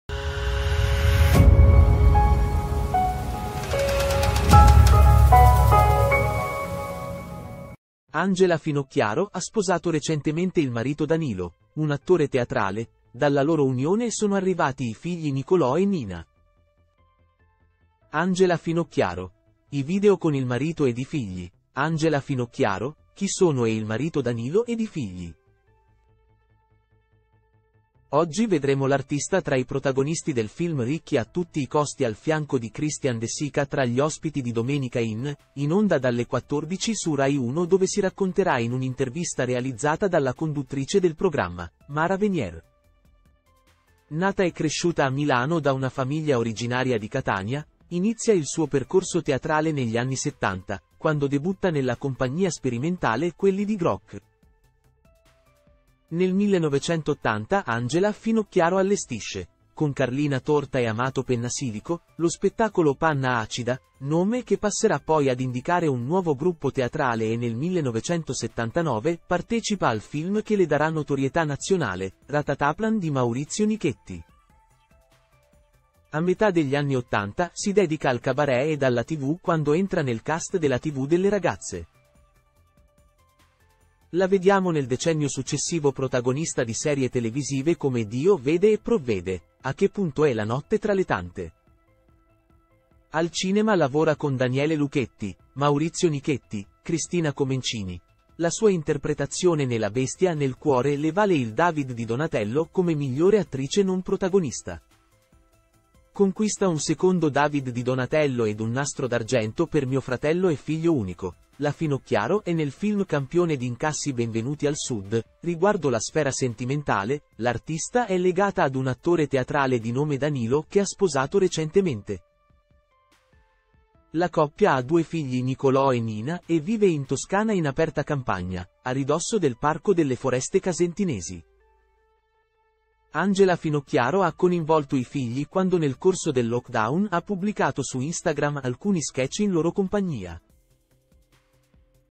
ANGELA FINOCCHIARO, CHI SONO E IL MARITO DANILO E I FIGLI
Angela Finocchiaro ha sposato recentemente il marito Danilo, un attore teatrale: dalla loro unione sono arrivati i figli Nicolò e Nina.

Angela Finocchiaro: i video con il marito ed i figli